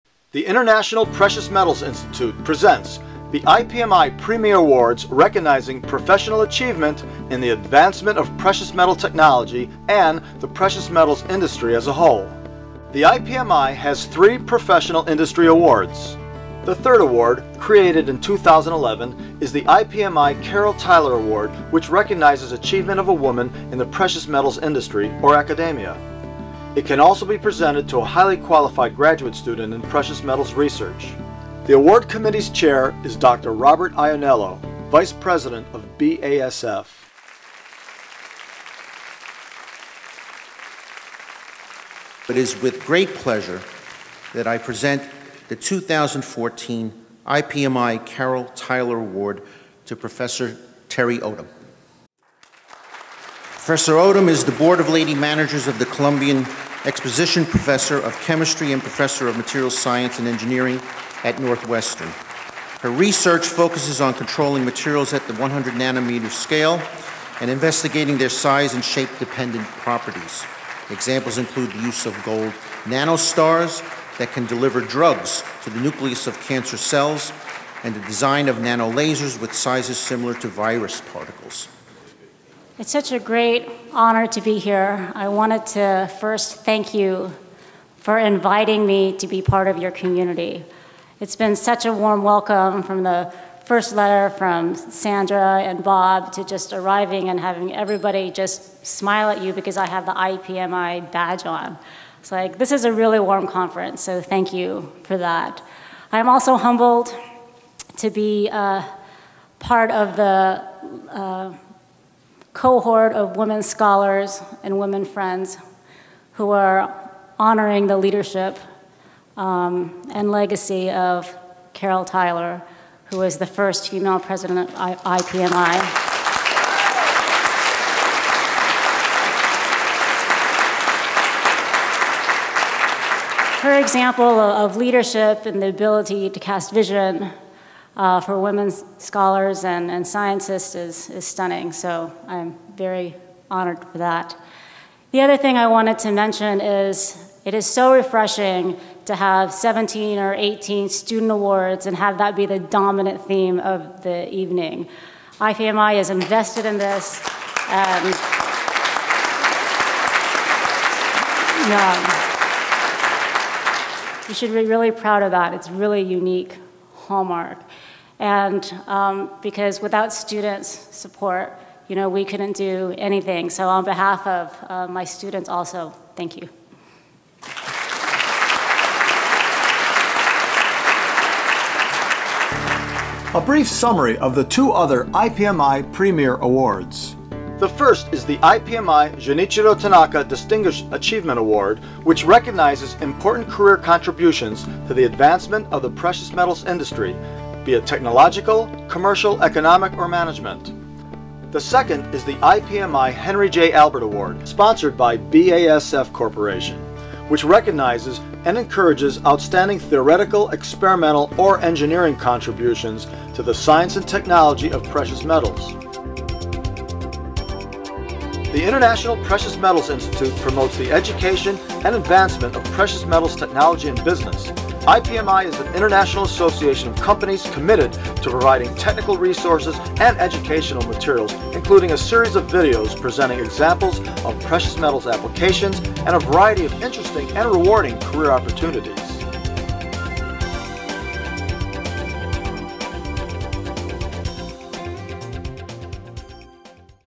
Teri W. Odom receives the 2014 IPMI Carol Tyler Award
Teri W. Odom is a Professor of Materials Science and Engineering at Northwestern University.  Her research focuses on controlling materials at the 100-nanometer scale.  Examples include the use of gold nanostars that can deliver drugs to the nucleus of cancer cells and the design of nanostars with sizes similar to virus particles.
The Carol Tyler Award recognizes the achievement of a woman in the precious metals industry and is presented by the IPMI.
IPMI.org